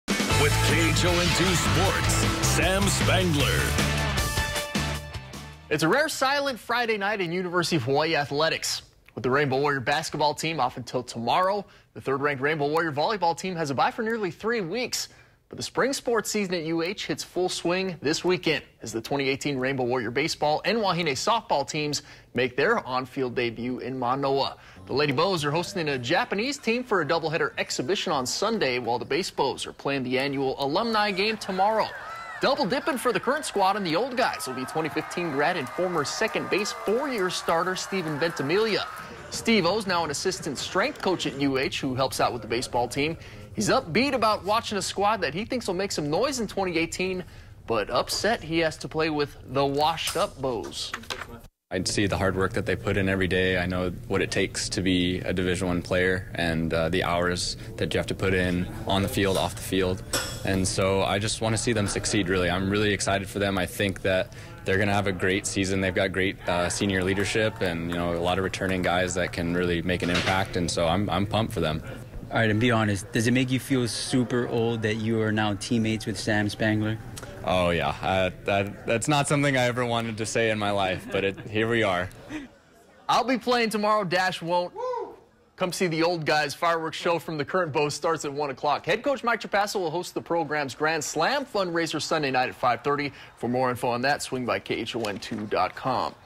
Base Bows to hold alumni game, Grand Slam fundraiser
HONOLULU — The University of Hawai'i baseball program will hold its 16th annual UH Baseball Grand Slam Celebration on Sunday, February 11 at the Stan Sheriff Center.